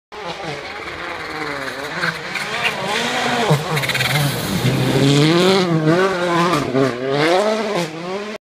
Sébastien OGIER on SS5 of Rally Sweden 2014
SS5, Kirkenær 2, Rally SWEDEN 2014